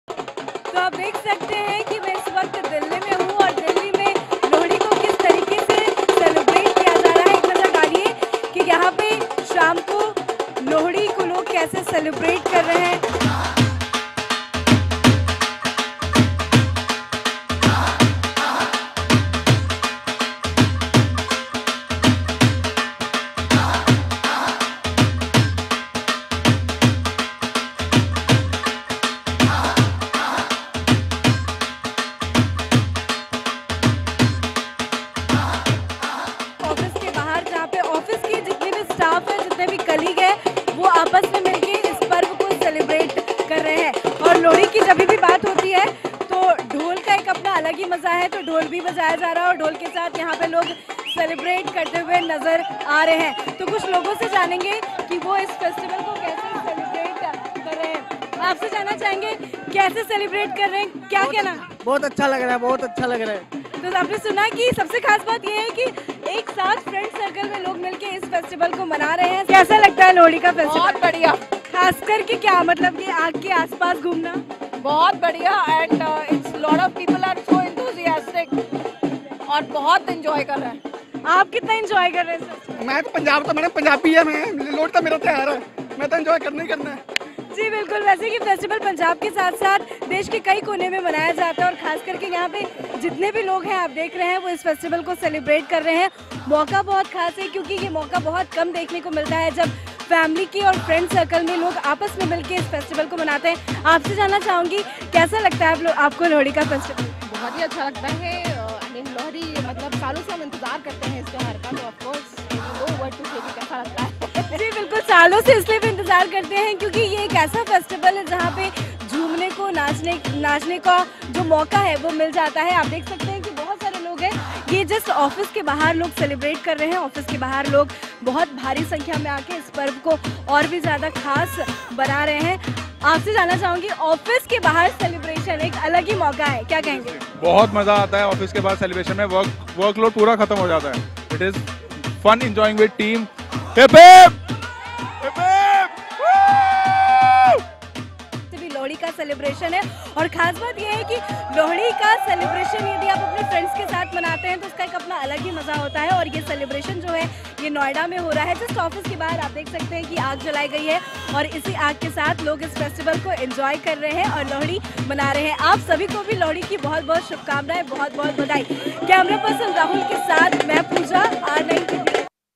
लोहड़ी की धूम, ढोल नगाड़ों की थाप पर झूमे लोग
देशभर में लोहड़ी का त्योहार बड़े ही धूमधाम से मनाया जा रहा है... नाच गाना ढोल नगाड़ों के साथ लोक इस पर्व को मना रहे हैं.. दिल्ली में भी लोहड़ी की रौनक देखी गई..